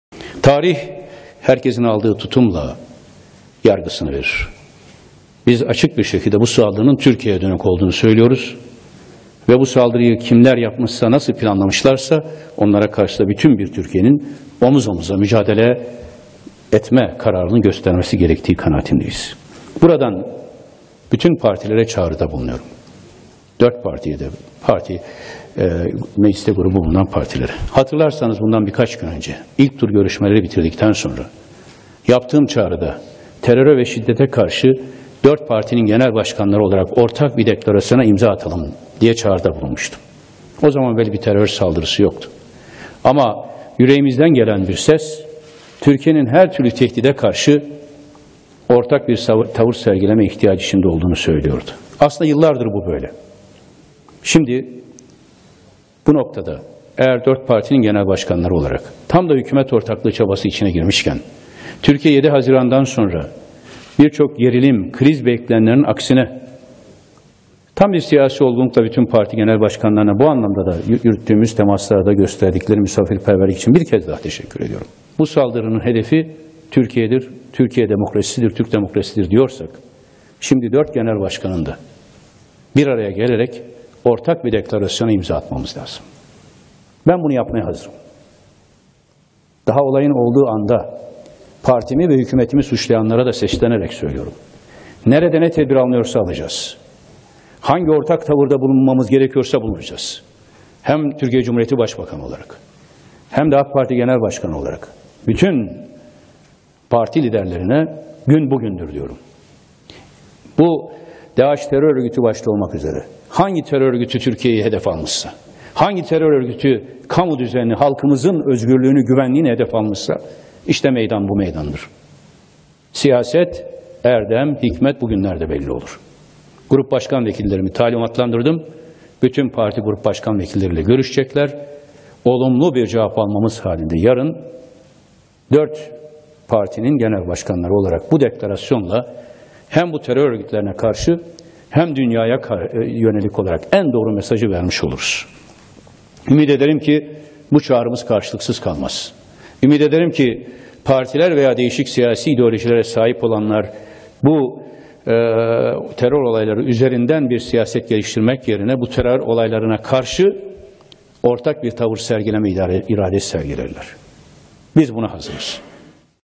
Davutoğlu'ndan Parti Liderlerine 'Suruç' Çağrısı
Suruç'taki patlamanın ardından Başbakan Ahmet Davutoğlu, 3 partiye teröre karşı yarın ortak deklarasyon yayınlama çağrısında bulundu.